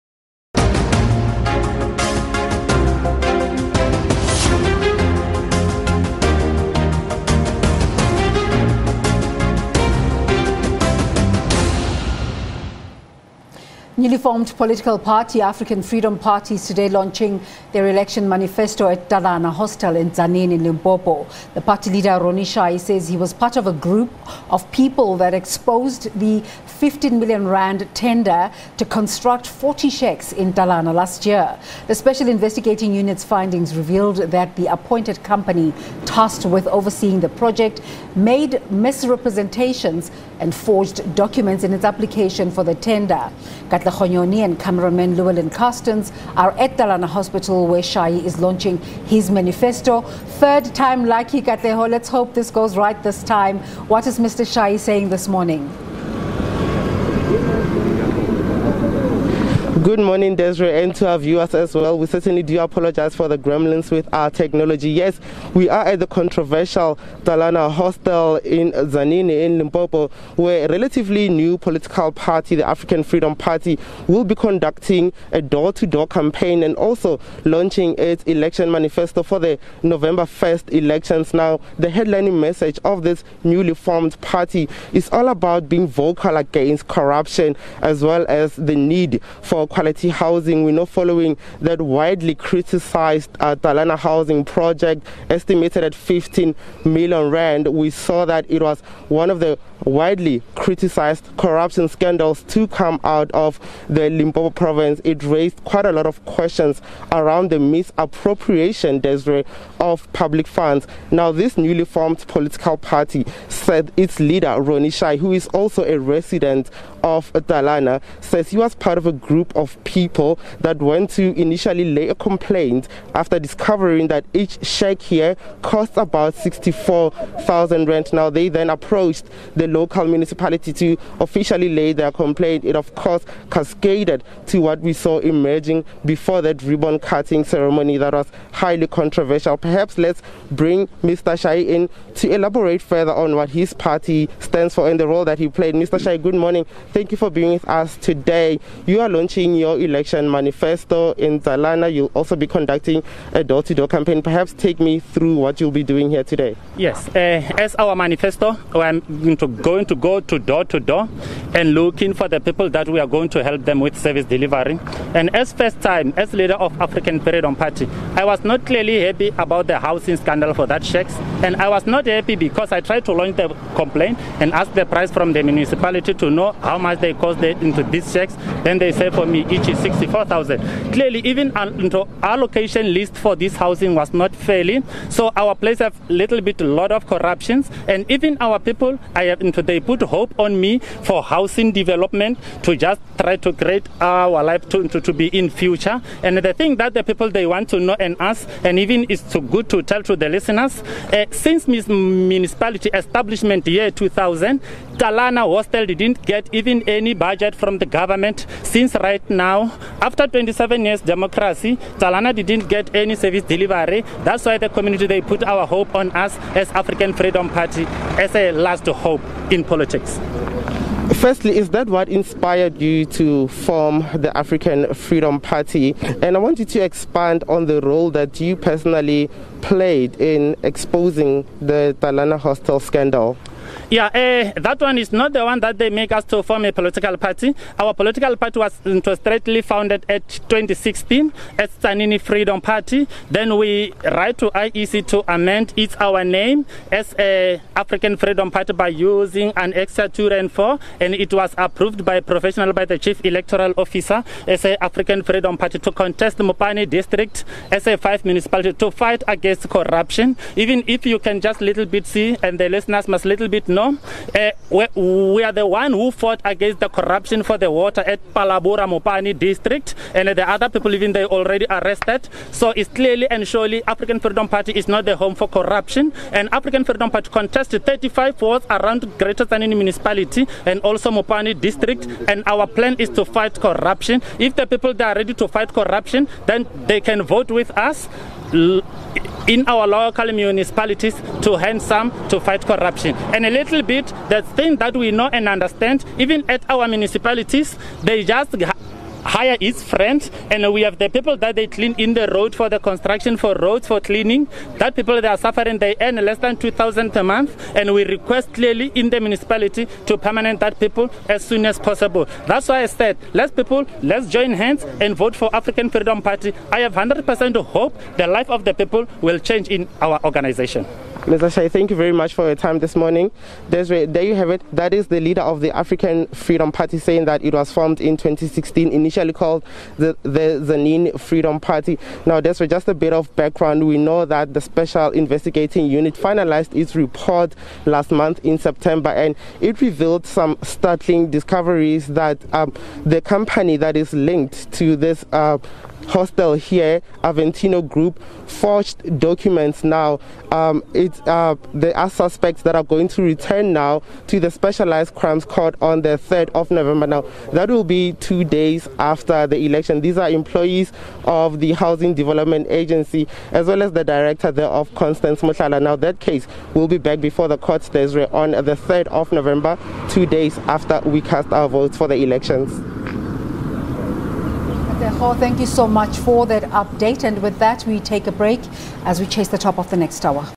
LGE 2021 | Newly-formed African Freedom Party launches manifesto in Limpopo
Newly formed political party, African Freedom Party is today  launching its election manifesto at Talana Hostel in Tzaneen, Limpopo. Party leader Ronny Shai says he was part of a group of people that exposed the R15 million tender to construct 40 shacks in Talana last year. The Special Investigating Unit's findings revealed that the appointed company tasked with overseeing the project, made misrepresentations and forged documents in its application for the tender.  Katlego Nyoni, and cameraman Llewellyn Carstens are at Talana hostel where Shai is launching his manifesto.